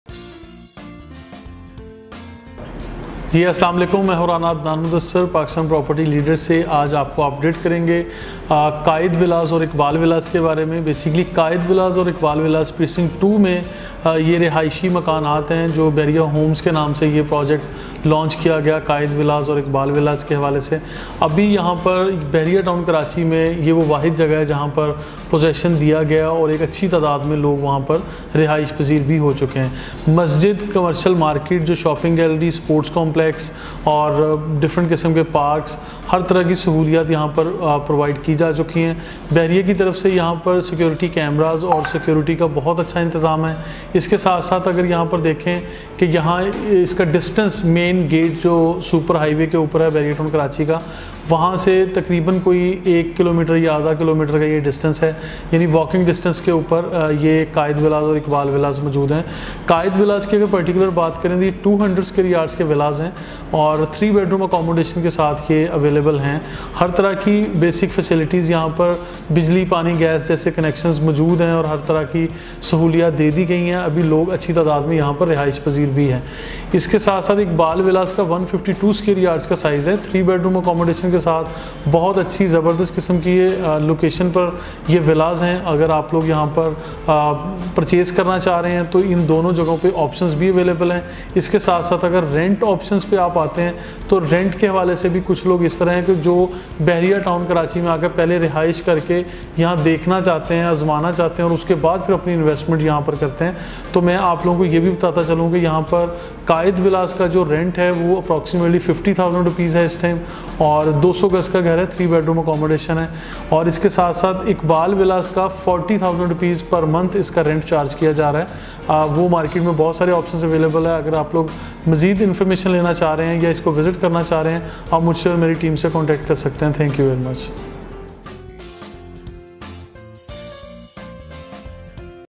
Precinct 2 : Quaid Villas & Iqbal Villas
Bahria Town Karachi is wonderful place to live and invest. No one can deny from its importance and reality. Pakistan Property Leaders (PPL) offers you best opportunity to live in Bahria Town Karachi.
Quaid vIllas and Iqbal villas are best option to live and enjoy.Iqbal villas
3 Bedrooms accommodation
152 square yards
Walking distance from main gate
Quaid villas
3 Bedrooms accommodation
200 square yards
 Near to main gate and Precinct 1
Attractions of Precinct 2
Iqbal villa & Quaid villa

Masjid & Parks

Shopping Gallery

Sports complex

Healthy life style

24/7 Security System

Security Cameras are installed on every single street corners

Schools & Hospital nearby